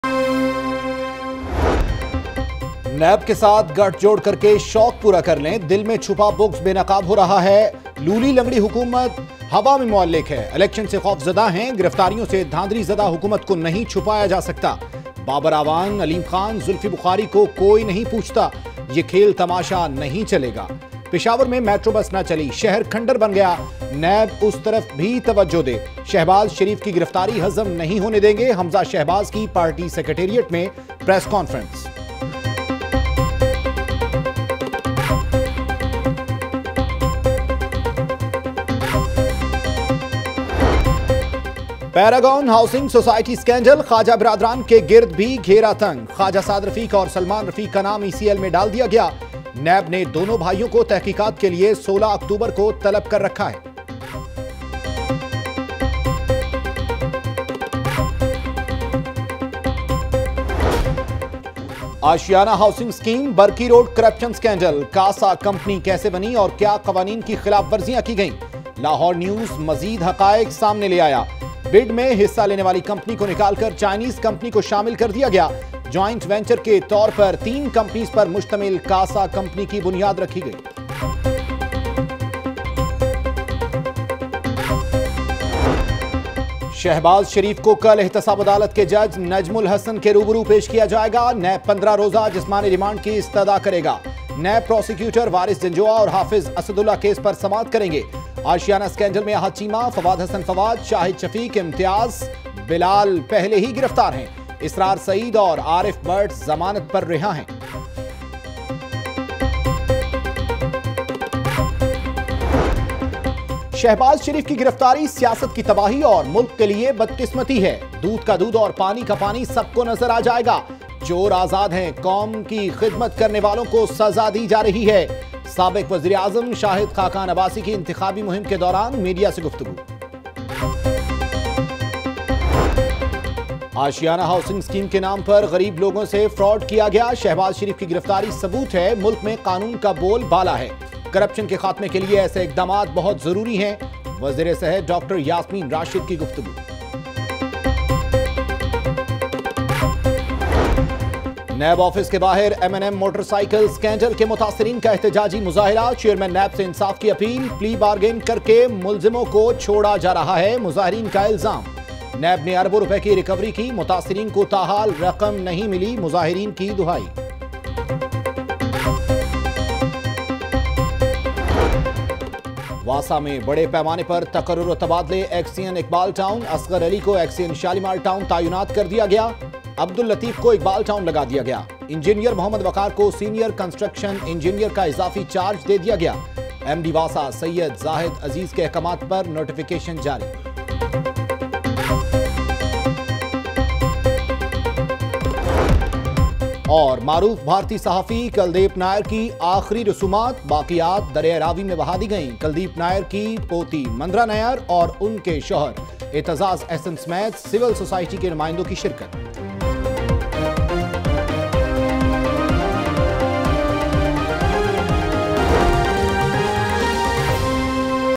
08 PM Headlines Lahore News HD – 05 October 2018
Watch Latest News: Shehbaz Sharif Arrest Nab, Mehmood ur Rasheed’s son charged for violence on police officials, Fight breaks out among PTI and PML-N members in Factory Area, PPP files petition seeking suspension of CM Punjab Usman Buzdar, Rana Sanaullah declared mastermind of Model Town massacre, Arrest warrant of Former IG Punjab Mushtaq Sukhera issued, Rana Mashood responds to controversial statement, Shahbaz Sharif angry on Rana Mashood over his interview, Dr Shahzad Naseem wins by-polls on Senate’s seat, Hamza Shahbaz criticizes PM Imran Khan, Khawaja Saad Rafique blames PTI for price hikes, ICC World Trophy comes Lahore , Police officials torture innocent lady, Scuffle breaks out between locals and traffic wardens, LPG prices witness sharp increase, Oil tanker collides at Waris Meer underpass and much more.